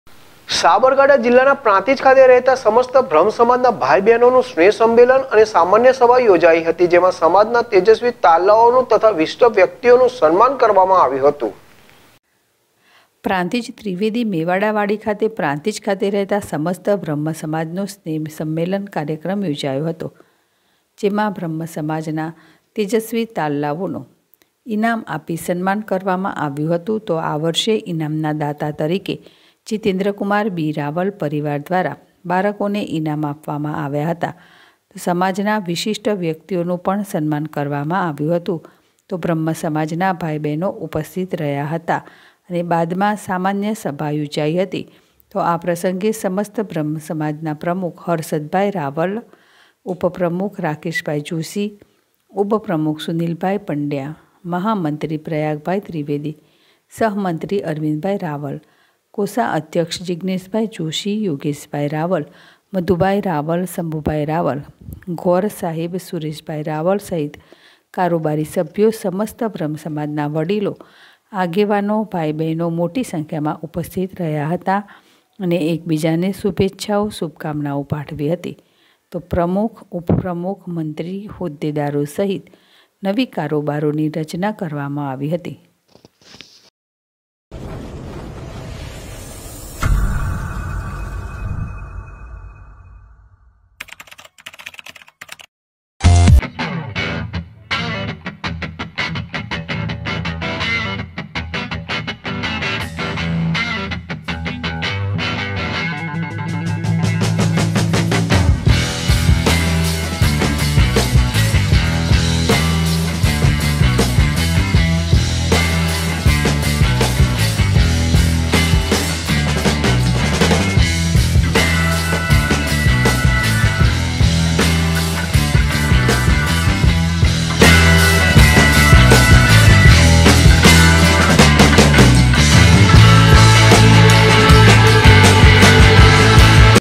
પ્રાંતિજ ખાતે રહેતા સમસ્ત બ્રહ્મ સમાજ ના ભાઇઓનું સ્નેહ સંમેલન યોજાયુ
પ્રાંતિજ ખાતે રહેતા સમસ્ત બ્રહ્મ સમાજ ના ભાઇઓનું સ્નેહ સંમેલન યોજાયુ/ZSTVNEWS.//